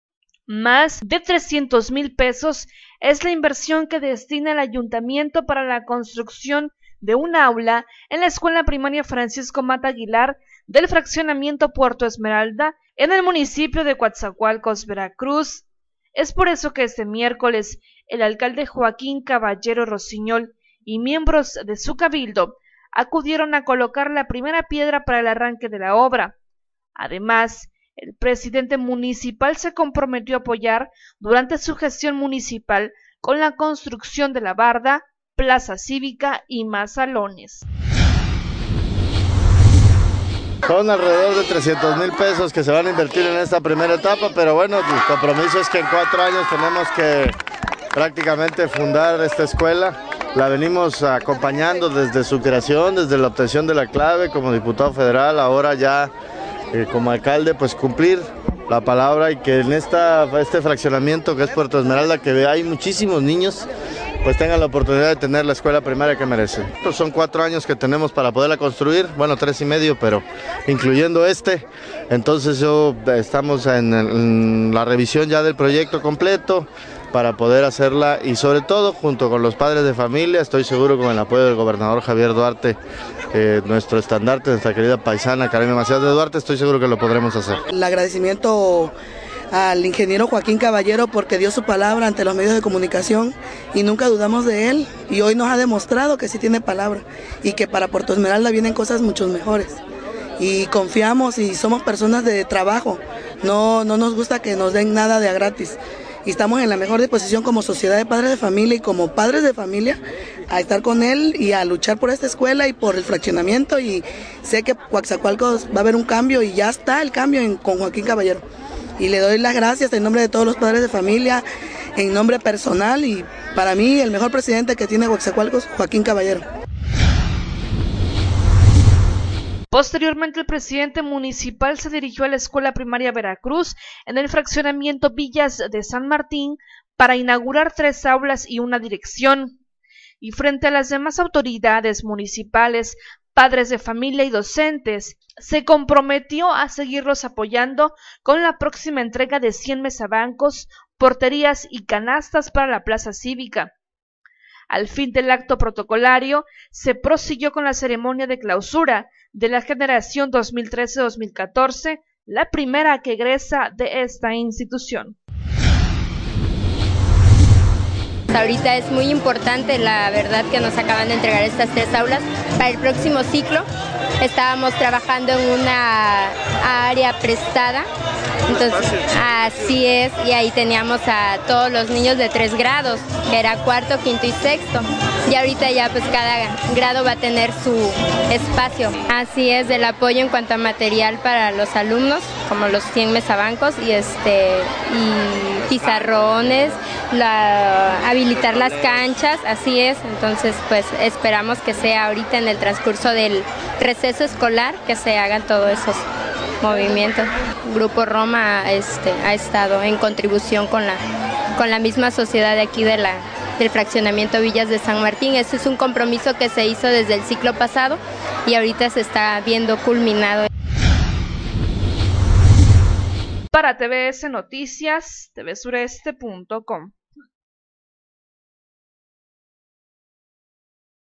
TVS Noticias.- Alcalde de Coatzacoalcos arranca construcción e inaugura aulas en escuelas
- Alcalde de Coatzacoalcos arranca construcción e inaugura aulas en escuelas